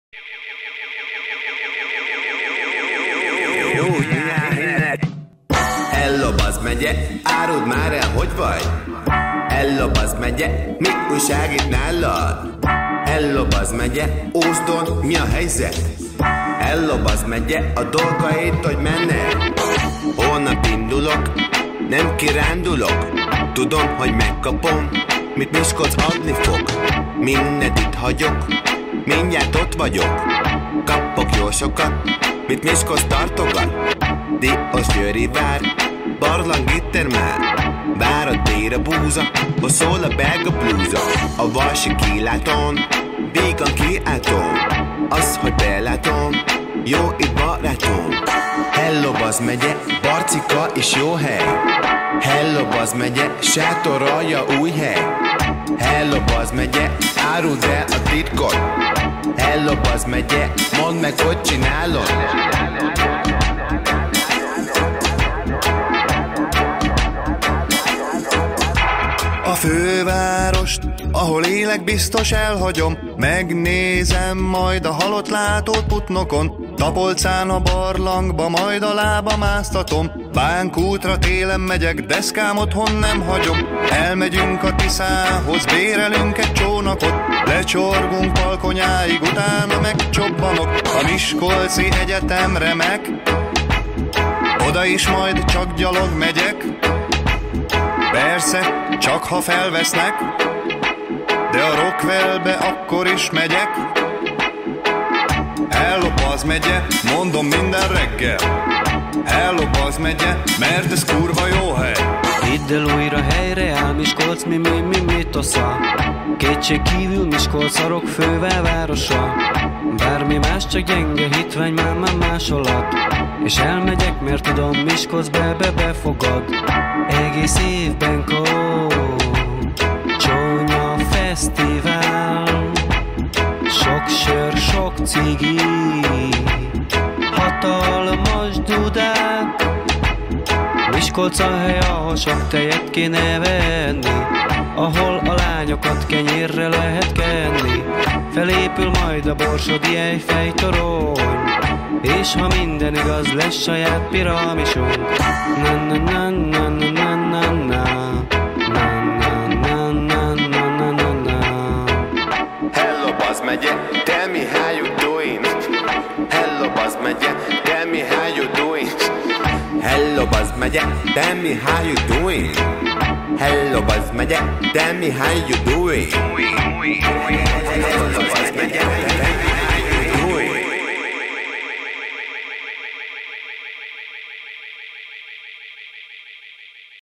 Kocsonyafesztivál 2011
A 2011-es Kocsonyafesztivál rövid összefoglalója.